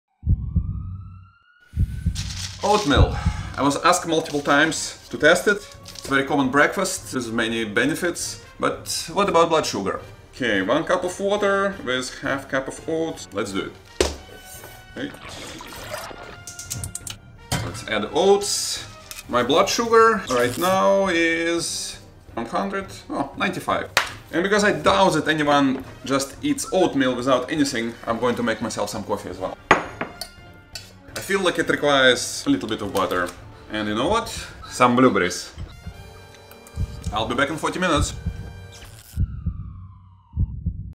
is Oatmeal Silently Killing You? 😮 (Blood Sugar Test)
In this round we look at oats. Oatmeal is a typical "healthy" breakfast choice.
But it can easily turn into a carb overload, if not balanced with a good dose of protein. `Behold!

Mind you it's all an N of 1 experiment. But if you suspect any metabolic issues, which are very common, you should consider test yourself with a blood glucose monitor.

1) Small, 80/20 (easy, but smart) consistent lifestyle changes and
2) Banding together to support each other and to cause ripple through scientific and political scene.